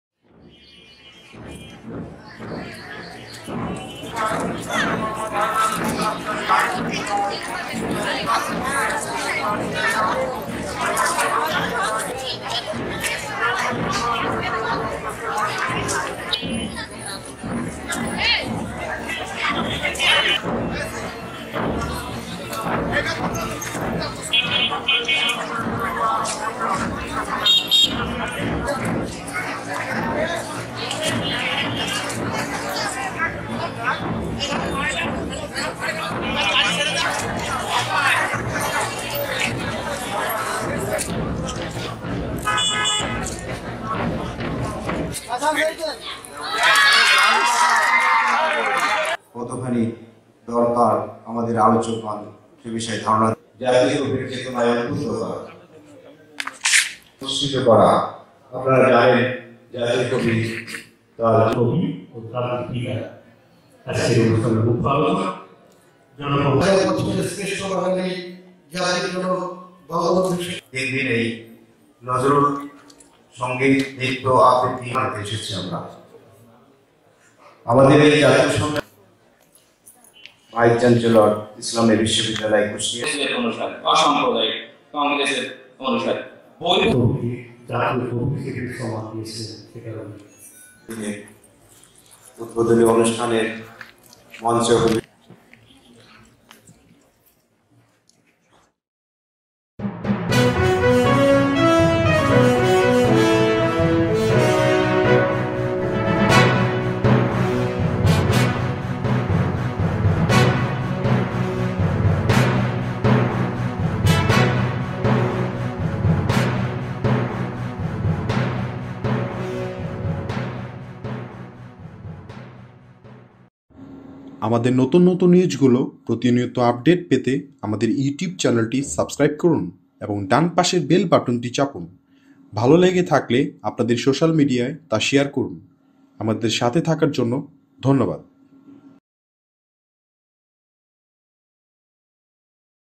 ৩ দিন ব্যাপী জাতীয় নজরুল সম্মেলন শুরু || 3 din bepi jatio nozrul sommelon shoro
ঝিনাইদহে শুরু হয়েছে ৩ দিন ব্যাপী জাতীয় নজরুল সম্মেলন। এ উপলক্ষে শনিবার শহরের পুরাতন ডিসি কোর্ট চত্বর থেকে একটি র‌্যালী বের করা হয়।